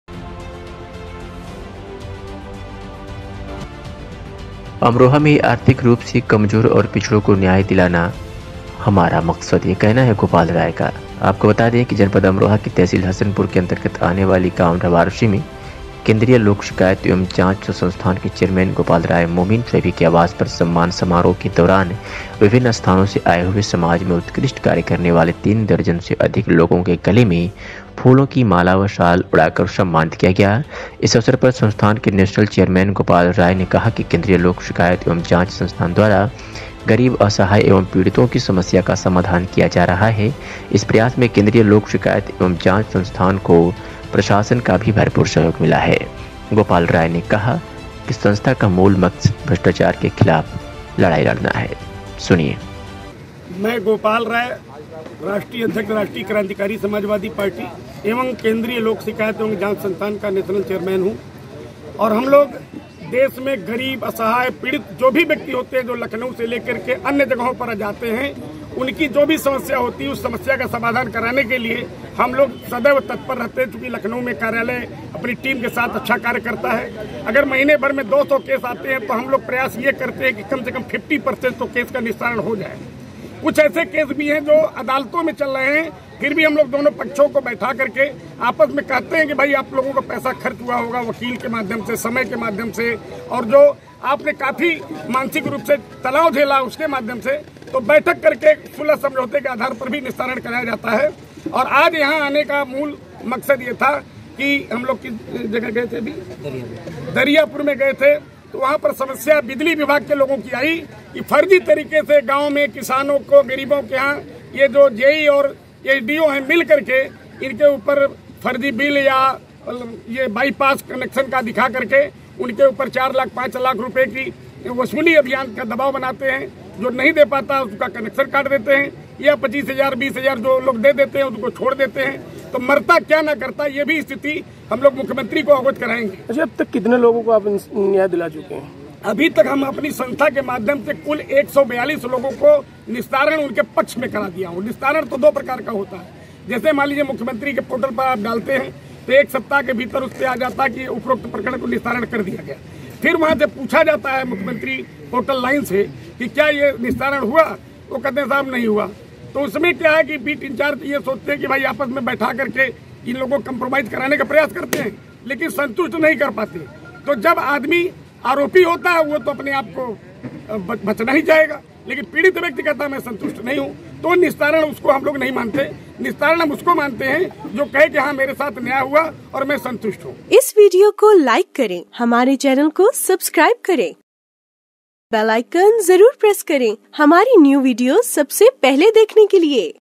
||Amroha|| आर्थिक रूप से कमजोर और पिछड़ों को न्याय दिलाने के लिए अब इस बड़े नेता ने उठाई आवाज़
आवश्यक सूचना -